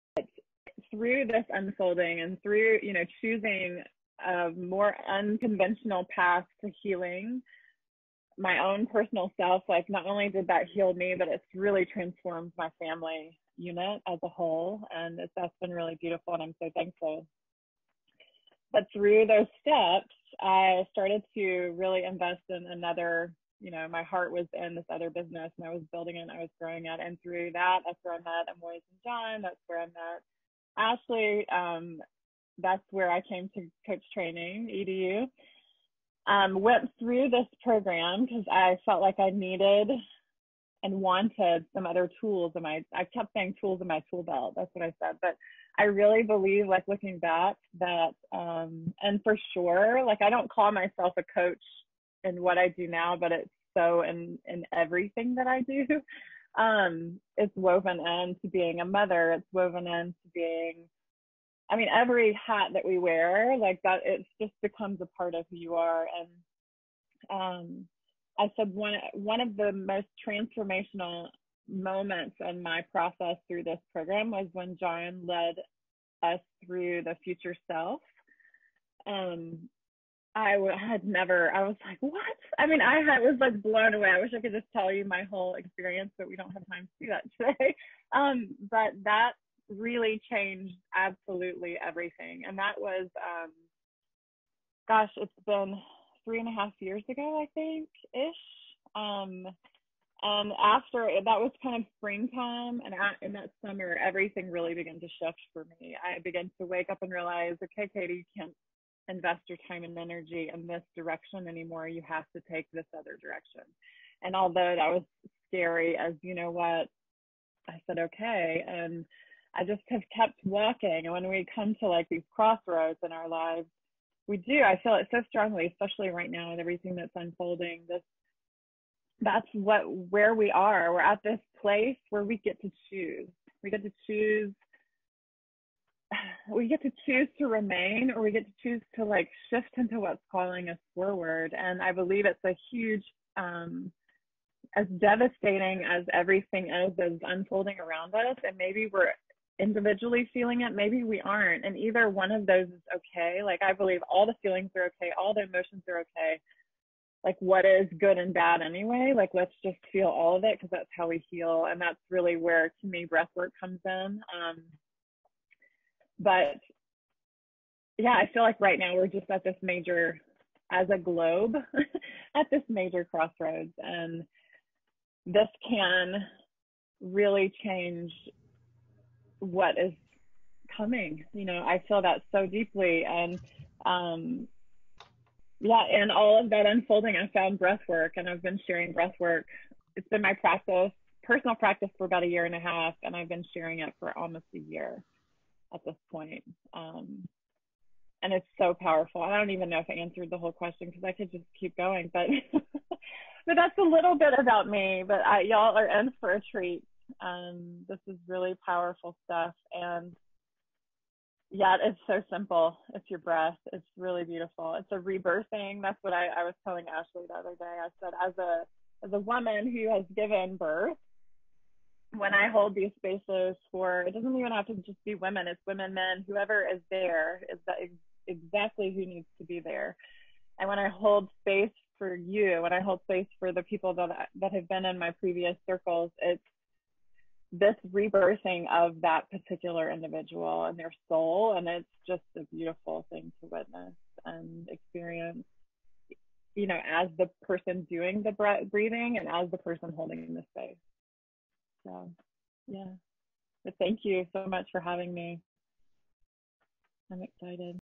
Coaching to Flourish | Breathwork Introduction with Katie Schomberg
Learn more about CTEDU Graduate Katie Schomberg and how she found coaching and breathwork.